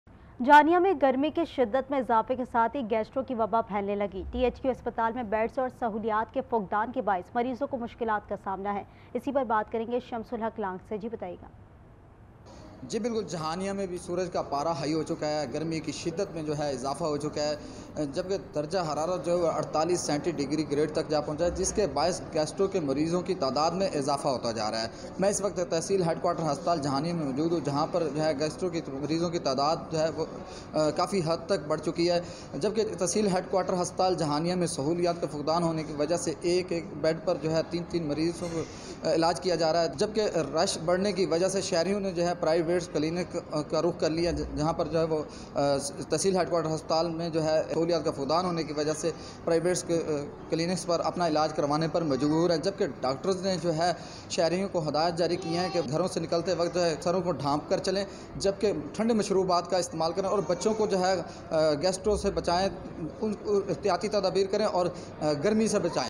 Jahanian: Lack of facilities in THQ Hospital, patients worried - Aaj News
Jahanian: Lack of facilities in THQ Hospital, patients worried - 20 May, 2024  - Aaj News

Aaj TV Official:
shorturl.at/iBDNX